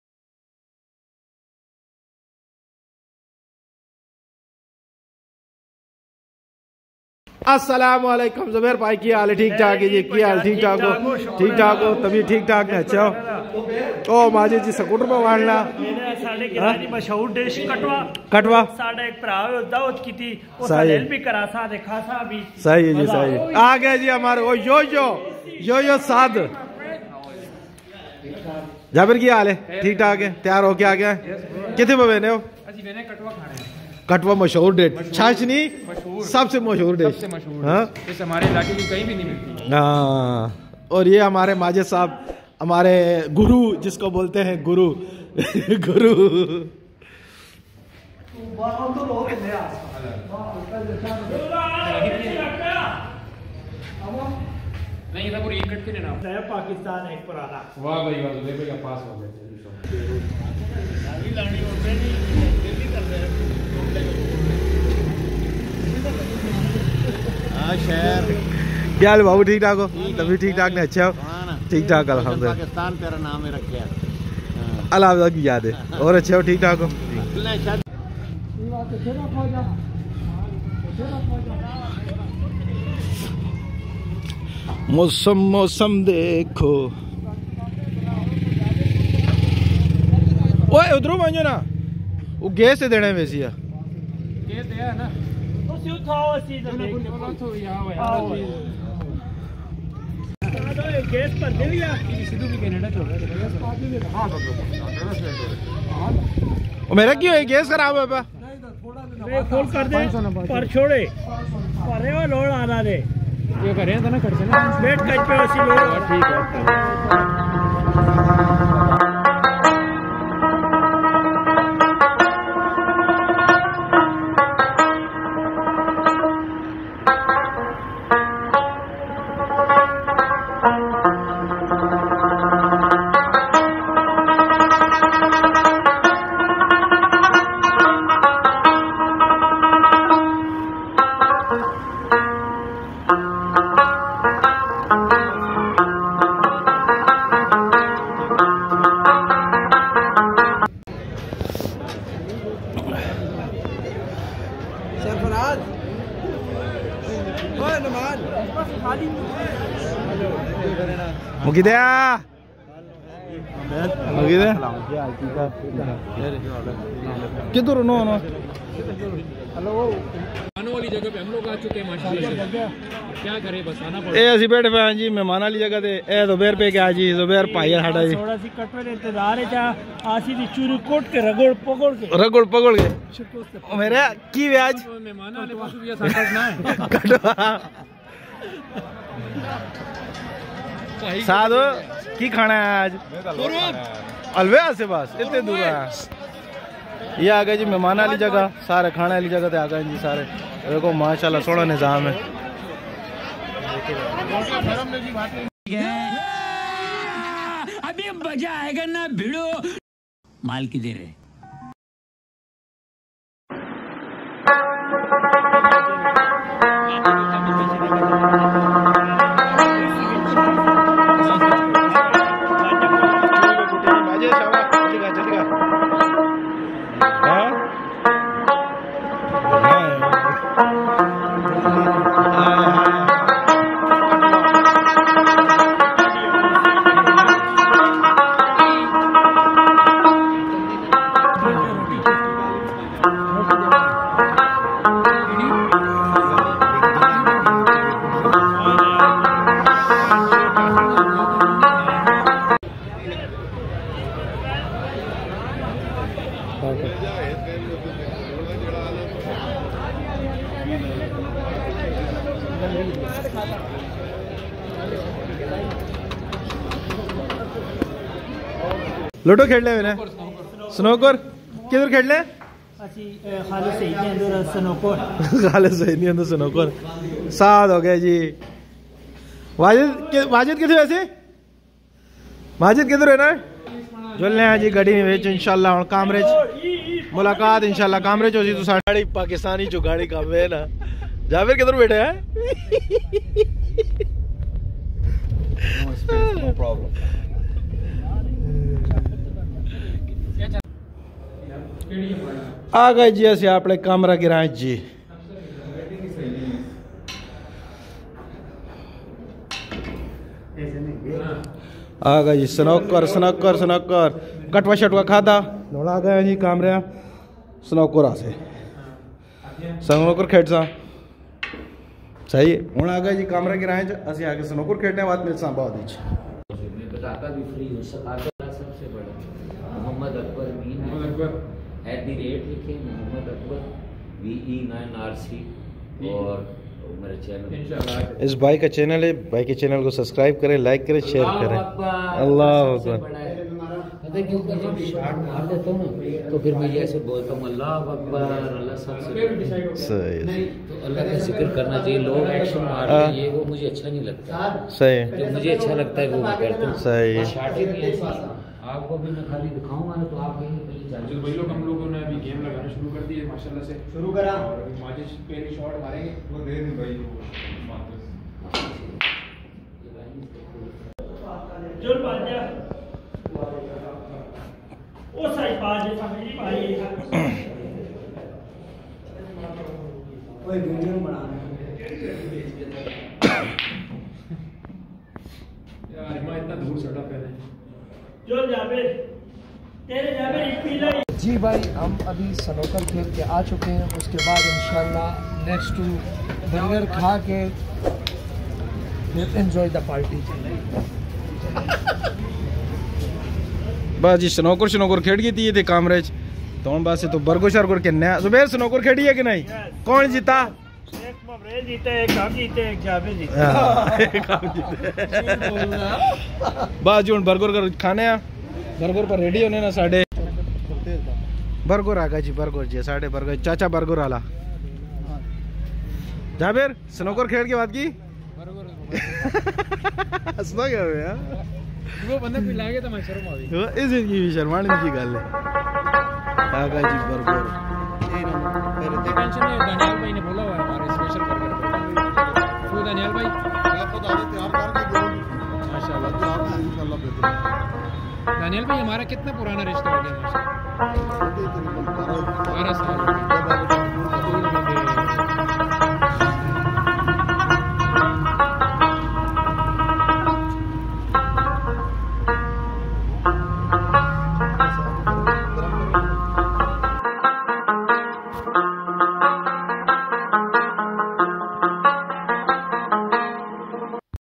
Attock Famous Katwa Gosht Ki Dawat&Halwa/ VillageWeddingProgram
Attock Famous Katwa Gosht Ki Dawat & Halwa/ Village Wedding Program / First Time Snooker Kheli.